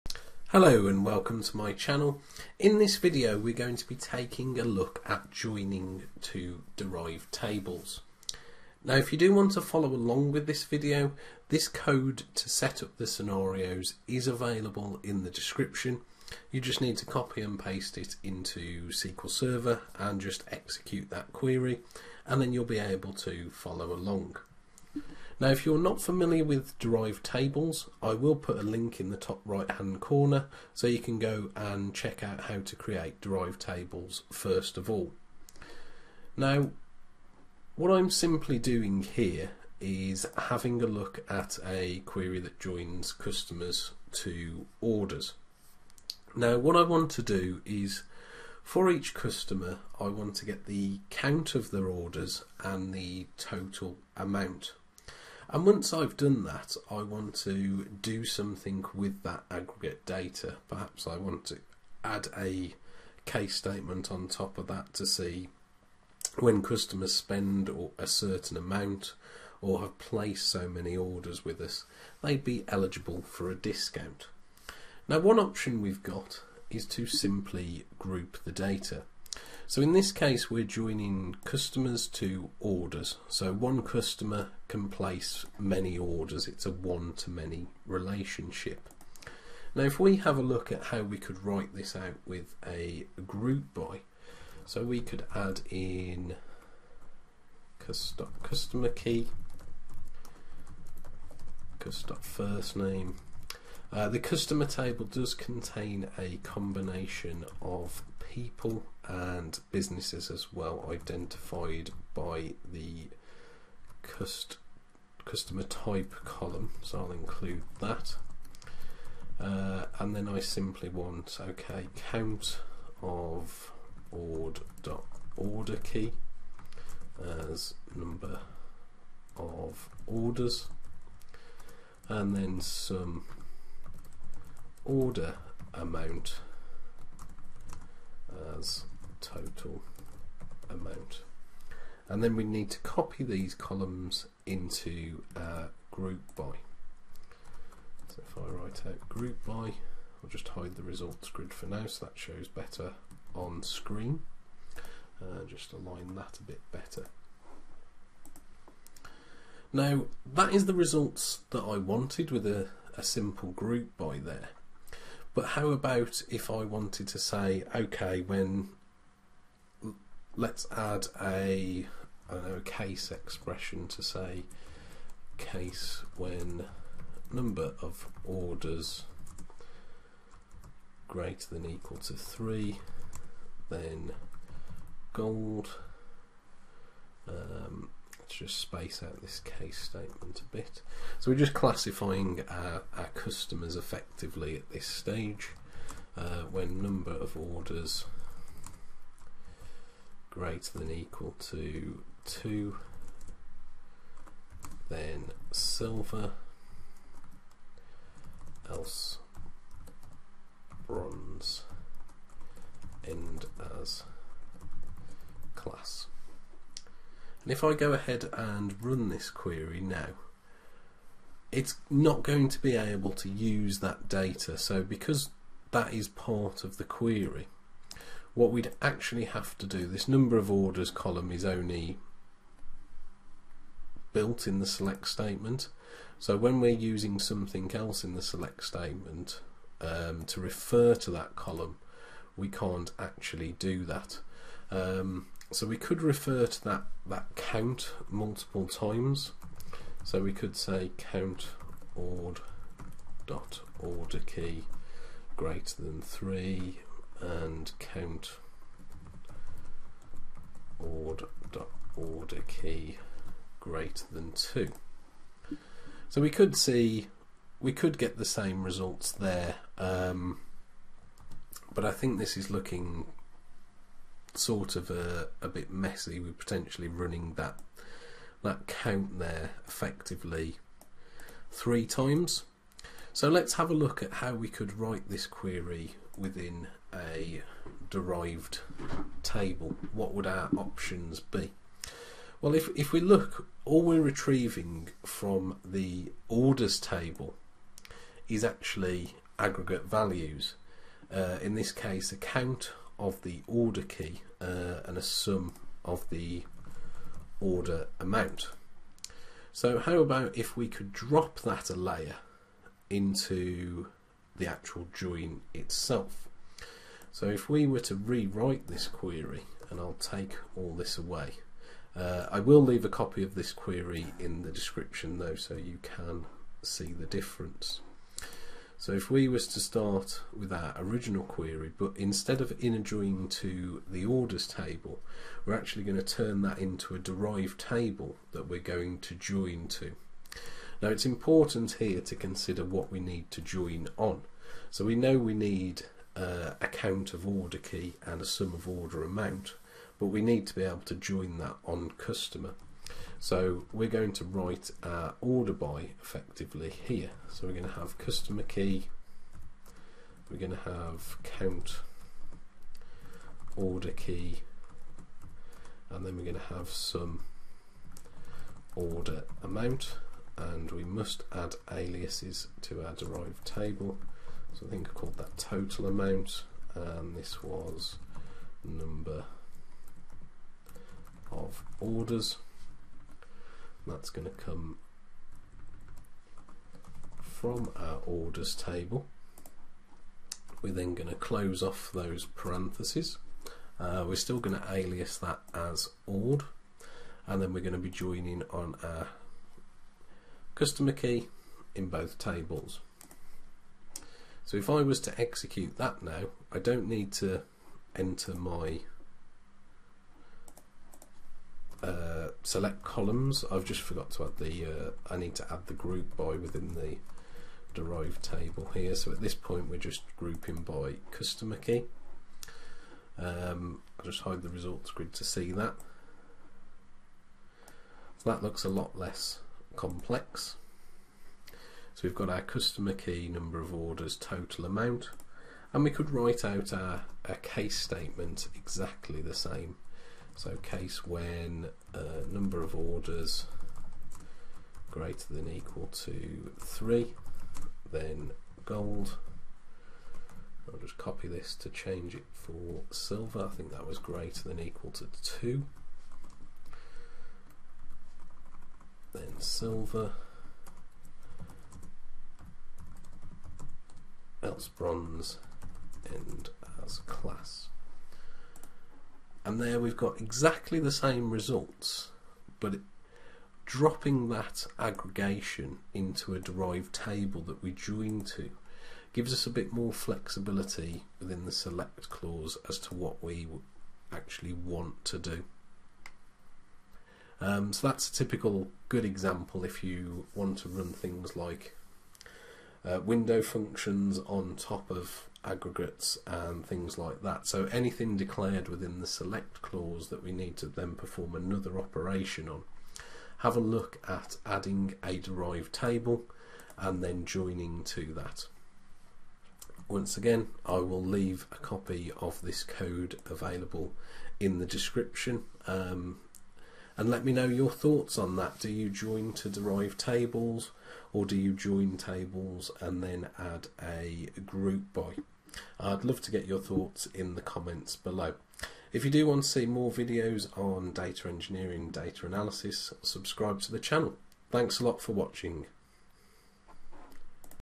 SQL Tutorial - How to Join to Derived Tables
In this SQL Tutorial learn to how to join to derived tables.  I talk you through the benefits of derived tables, show you to create a derived table then how to join to the derived table.  In the video we work through practical examples, creating the same query with a join between two tables then grouping and creating a derived table then joining to give the same results.

If you are not familiar with derived tables then you can check out this video on my channel

If you would like to follow along with the video you can use the code examples below:
CREATE TABLE dbo.Customers
 CustomerKey INT IDENTITY(1, 1) NOT NULL
  CONSTRAINT PK_Customers_CustomerKey PRIMARY KEY (CustomerKey),
 FirstName VARCHAR(50) NULL,
 LastName VARCHAR(50) NULL,
 BusinessName VARCHAR(50) NULL,
 CustomerType CHAR(1)

INSERT INTO dbo.Customers (FirstName, LastName, BusinessName, CustomerType)
VALUES
 ('Albert', 'Gunner', NULL, 'P'),
 (NULL, NULL, 'Beach Store', 'B'),
 ('Catherine', 'Smith', NULL, 'P'),
 (NULL, NULL, 'Duncan''s Hair', 'B'),
 ('Erin', 'Fairclough', NULL, 'P'),
 (NULL, NULL, 'Gaming Zone', 'B'),
 ('Henry', 'Long', NULL, 'P');

CREATE TABLE dbo.Orders
 OrderKey INT IDENTITY(1, 1) NOT NULL
  CONSTRAINT PK_Orders_OrderKey PRIMARY KEY (OrderKey),
 CustomerKey INT NULL,
 OrderDate DATE NULL,
 OrderAmount DECIMAL(8, 2)

INSERT INTO dbo.Orders (CustomerKey, OrderDate, OrderAmount)
VALUES

-- create query using GROUP BY
SELECT
 Cust.CustomerKey,
 Cust.FirstName,
 Cust.LastName,
 Cust.BusinessName,
 Cust.CustomerType,
 COUNT(OrderKey) AS NoOfOrders,
 SUM(OrderAmount) AS TotalAmount
FROM dbo.Customers AS Cust
INNER JOIN dbo.Orders AS Ord
 ON Cust.CustomerKey = Ord.CustomerKey
GROUP BY
 Cust.CustomerKey,
 Cust.FirstName,
 Cust.LastName,
 Cust.BusinessName,
 Cust.CustomerType;

-- create query using DERIVED TABLE
SELECT
 Cust.CustomerKey,
 Cust.FirstName,
 Cust.LastName,
 Cust.BusinessName,
 Cust.CustomerType,
 Ord.NoOfOrders,
 Ord.TotalAmount
INNER JOIN
 SELECT
  CustomerKey,
  COUNT(OrderKey) AS NoOfOrders,
  SUM(OrderAmount) AS TotalAmount
 FROM dbo.Orders
 GROUP BY
  CustomerKey
) AS Ord
 ON Cust.CustomerKey = Ord.CustomerKey;